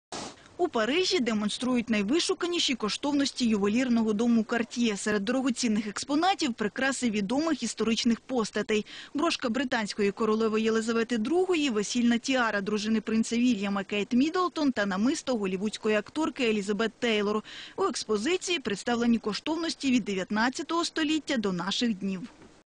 В Париже проходит выставка драгоценностей дома Cartier
В Париже проходит выставка драгоценностей дома Cartier - Подробности - Интер - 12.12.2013

In Paris, an exhibition treasures of the house of Cartier - Details - inter - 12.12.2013